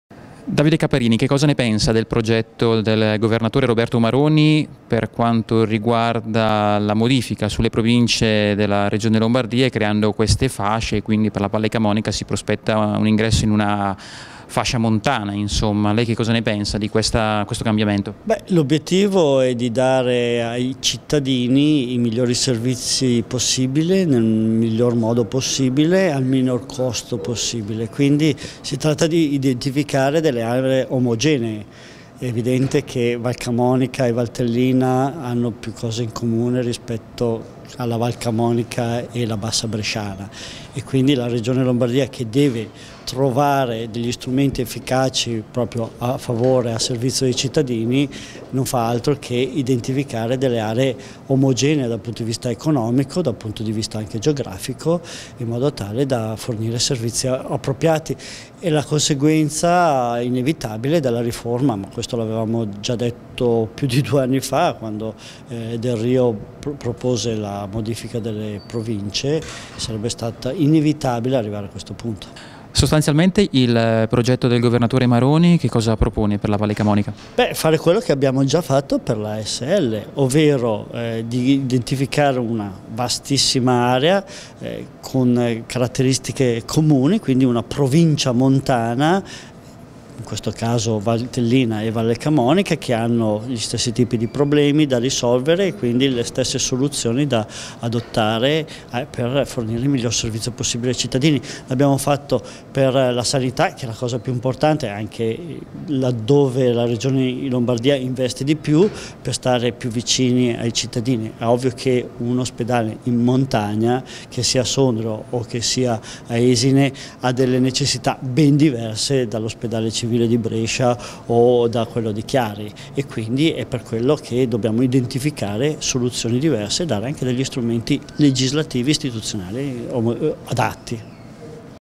Lombardia: Caparini, la priorità è tutelare i servizi ai cittadini
I dettagli tecnici, va detto, restano incerti: nell’area della montagna ci sarà la Valcamonica, ma Val Trompia e Val Sabbia? «Sono aree montane, ma per esempio la Valtrompia è legata per servizi e funzioni al capoluogo» fa notare il deputato leghista Davide Caparini. L’area omogenea metropolitana potrebbe invece non essere riservata a Milano, ma riguardare l’hinterland di tutte le città; per Brescia una sorta di giunta dei sindaci, disegnata attorno alla sp19. Oppure una fascia centrale, continua, da Milano a Brescia. Difficile dirlo oggi. Quel che è certo, spiega Caparini, è che «bisogna uscire dalla logica dei vecchi confini e identificare aree che hanno esigenze di gestione simili, omogenee: la montagna, le città, la Bassa». Una vecchia idea sostenuta dalla Lega. «È un po’ quello che si sta facendo con la riforma sanitaria regionale» dice Caparini.